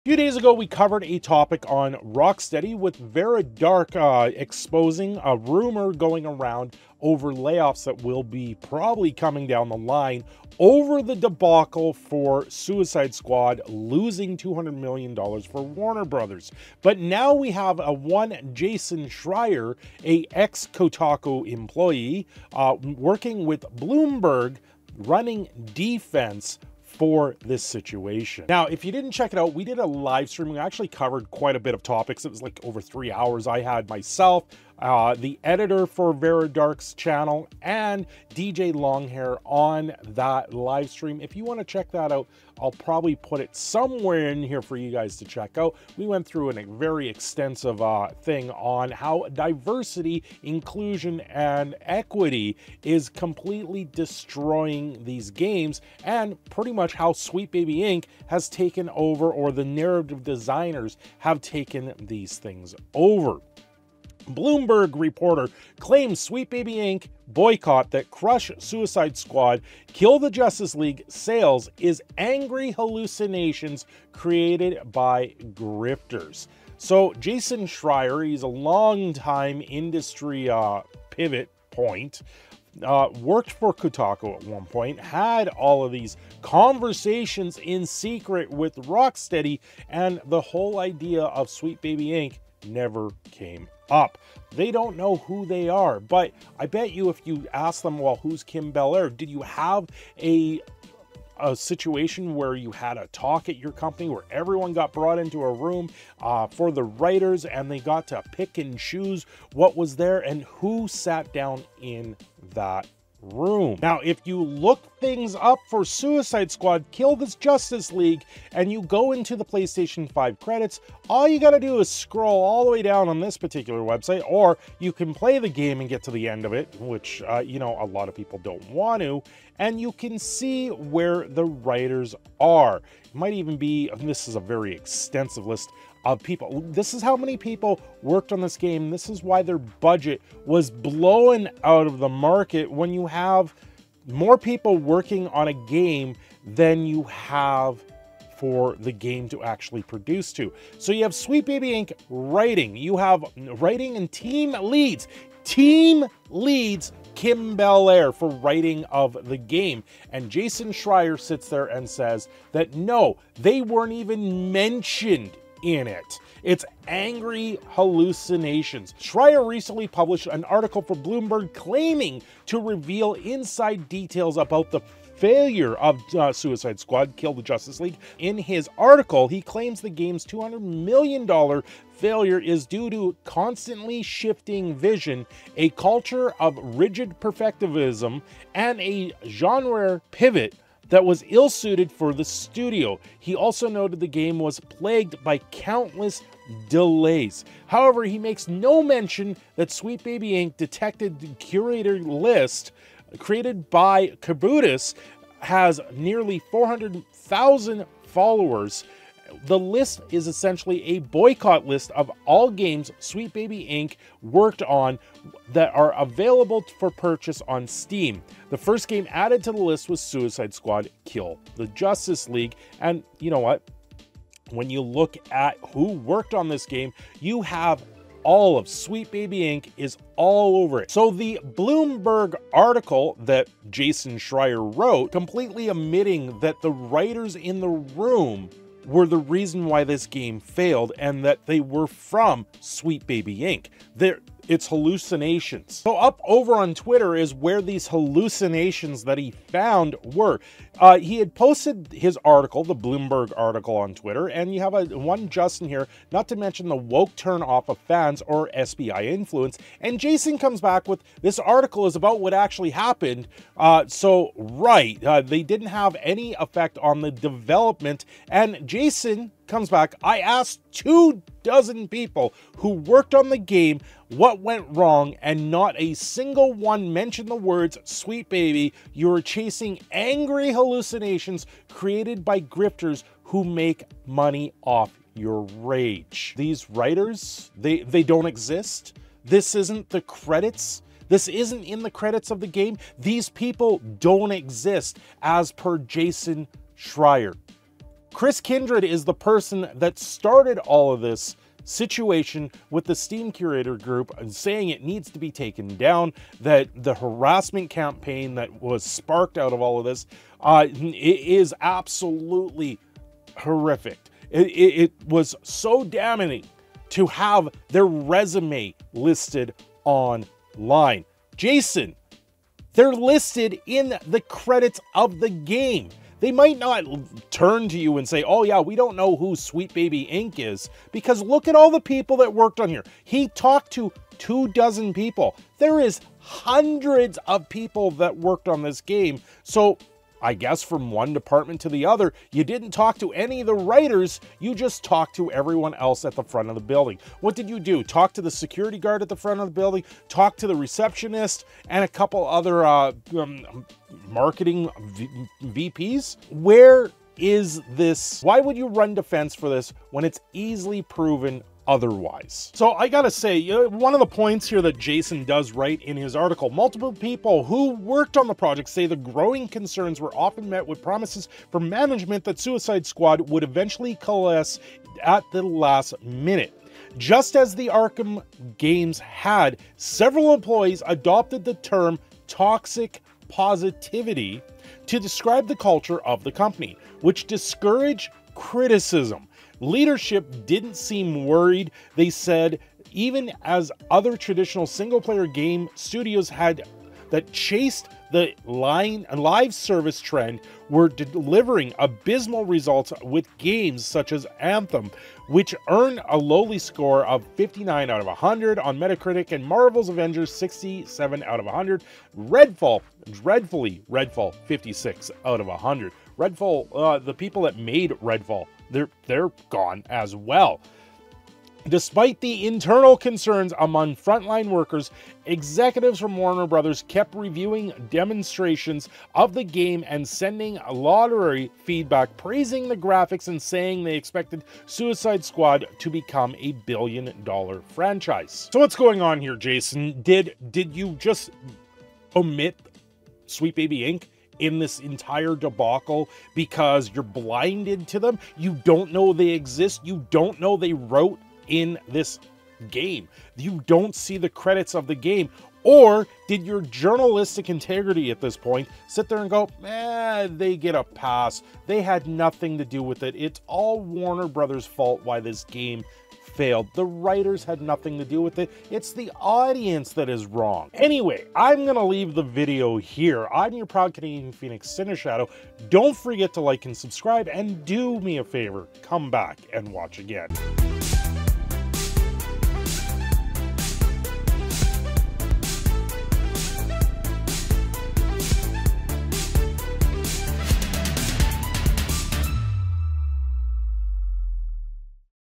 Former Kotaku Writer Jason Schreier Calls Sweet Baby Inc A HALLUCINATION Over Suicide Squad Failure
Jason Schreier, Former Kotaku Employee, now Bloomburg Writer calls Sweet Baby Inc Involvement with Suicide Squad Kill The Justice League a " hallucinations created by grifters"
Extra Life